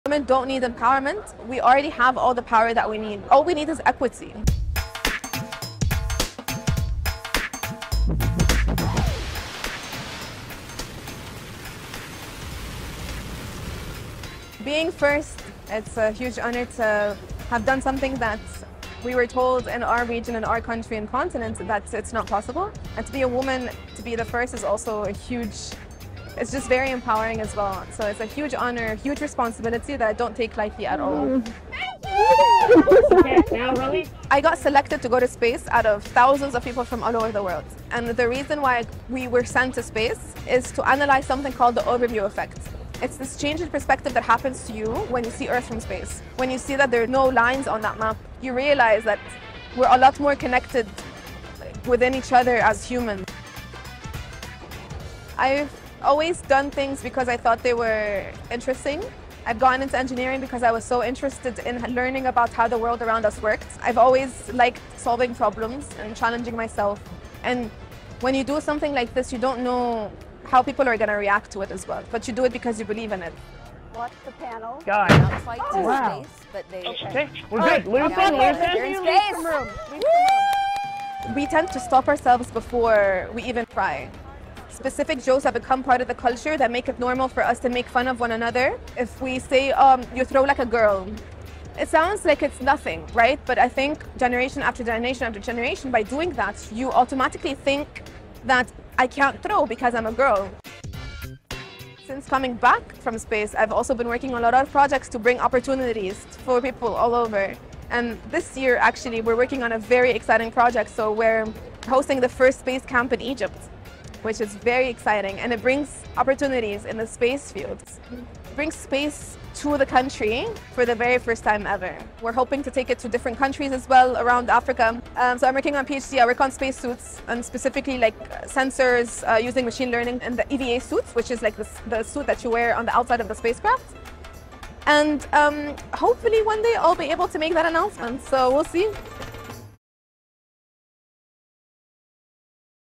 Inspiring Woman : अंतराळात जाणारी पहिली African Arab Astronaut सांगते, महिलांना समान वागणूक का हवी?
इजिप्तची सारा सार्बी अंतराळात पाऊल ठेवणारी पहिली आफ्रिकन आणि अरब महिला आहे. 31वर्षीय सारा पेशानं इंजिनियर आणि अंतराळप्रवासी आहे. अंतराळात वापरल्या जाणाऱ्या स्पेससूटवर ती संशोधनही करते आहे. बीबीसीसोबत बोलताना सारानं महिलांसमोरच्या अडथळ्यांवर आणि महिलांमधल्या क्षमतेवर भाष्य केलं.